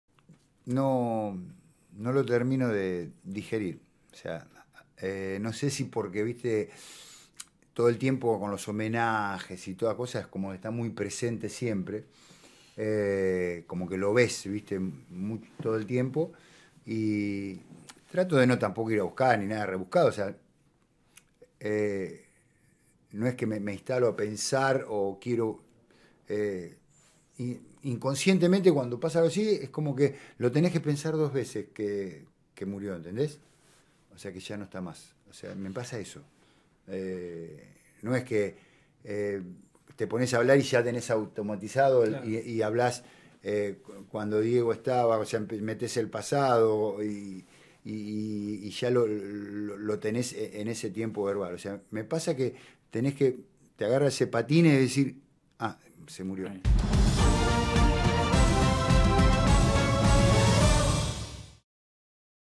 Sergio Goycochea: "No termino de digerir la muerte de Maradona" | 90+3 Podcast
Sergio Goycochea visitó los estudios de la CONMEBOL Libertadores en una charla imperdible. El querido ex arquero de la Selección Argentina también fue parte del plantel de River Plate campeón de la CONMEBOL Libertadores 1986.

En una amena conversación, Goyco recordó la figura de su amigo Diego Maradona y reveló lo difícil que es procesar su muerte.